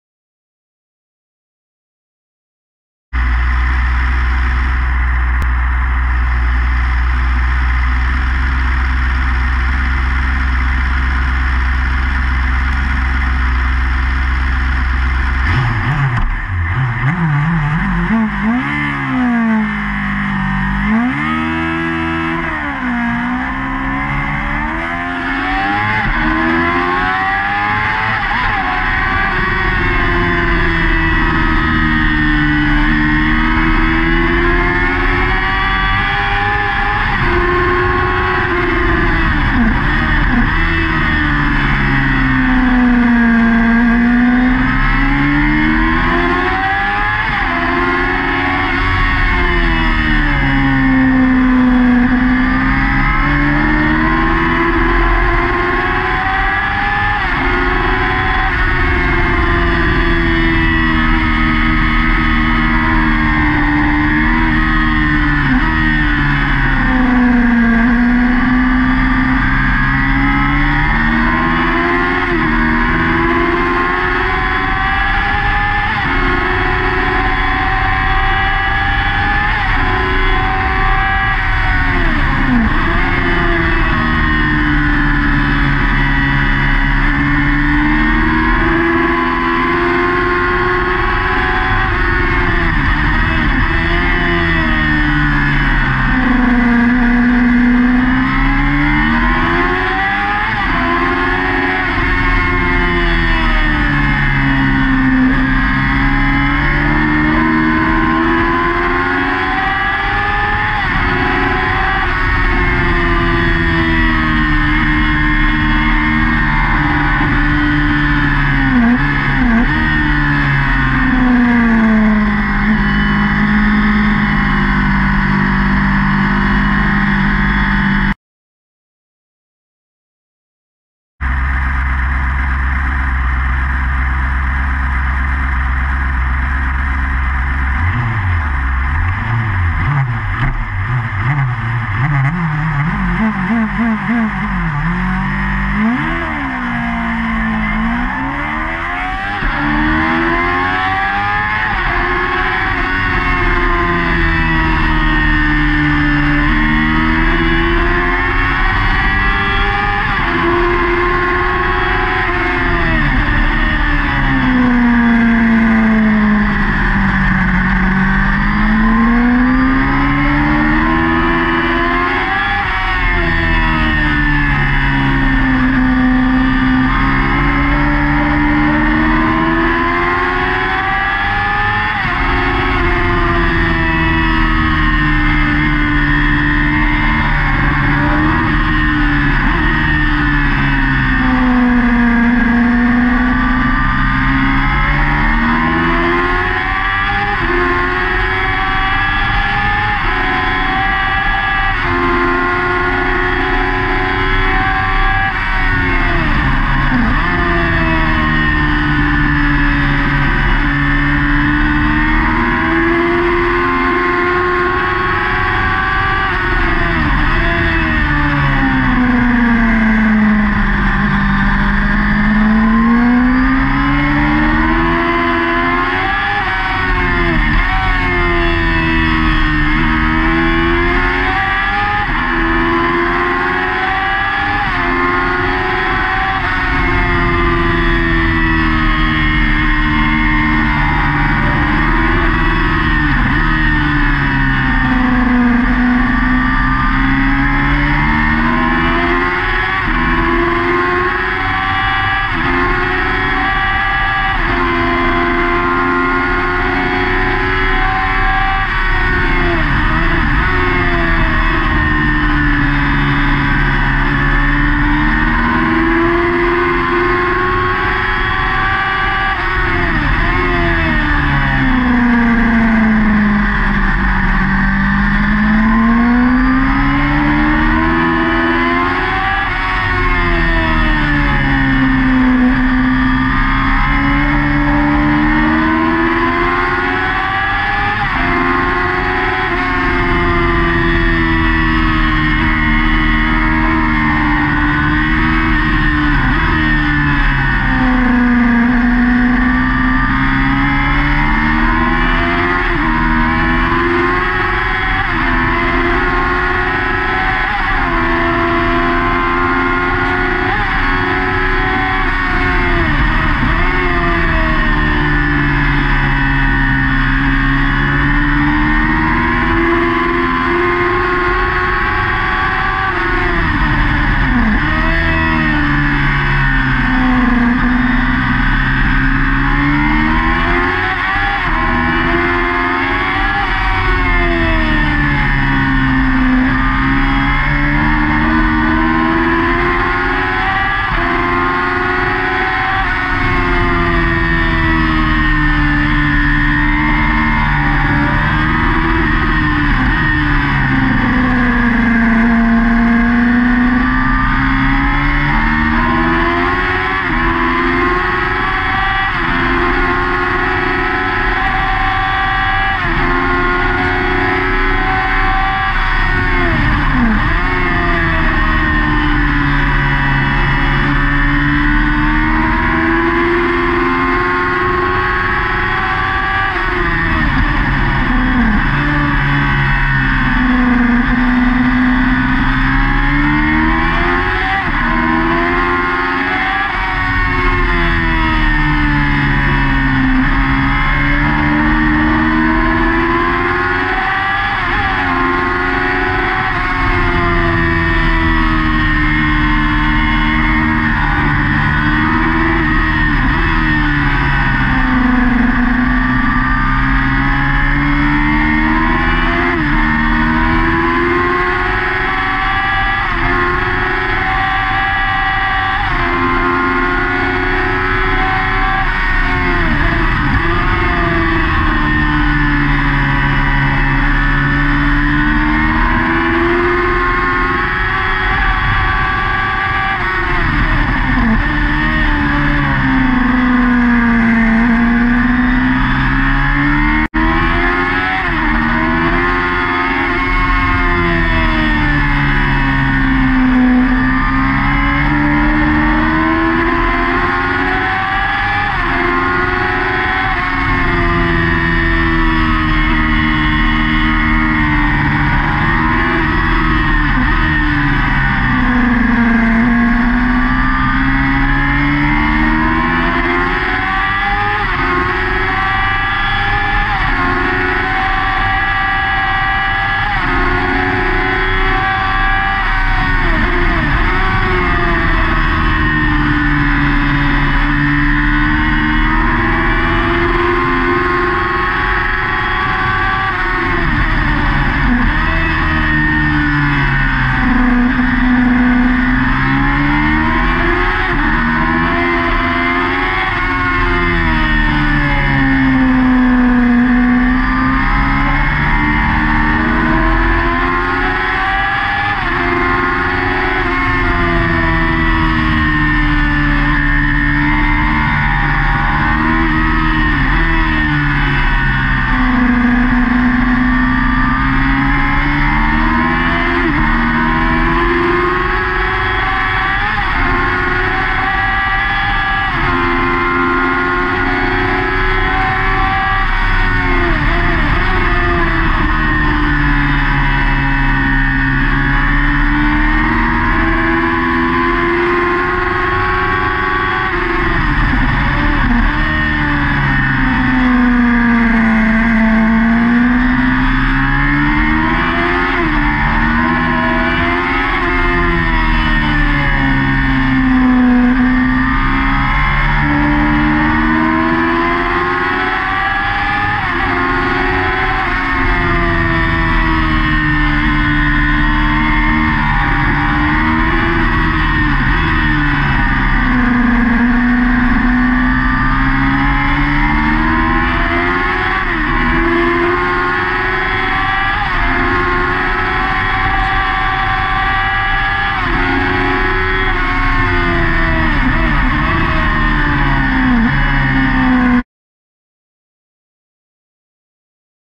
Thunderbike Sport Brands Hatch Indy Race 2
The second race from Round 1 at Brands Hatch Indy with BEMSEE.
Race 2 (Groups A&C) of the Thunderbike Sport Championship onboard Tom Watson, Tea Time Racing Yamaha R6, supported by EBC Brakes, Torq Fitness, North East South Wherever and MotorsportDays.com
Dan Singleton won the race followed by Michael Simmons and myself. The race was rerun following an unfortunate incident for a fellow competitor at Paddock Hill bend.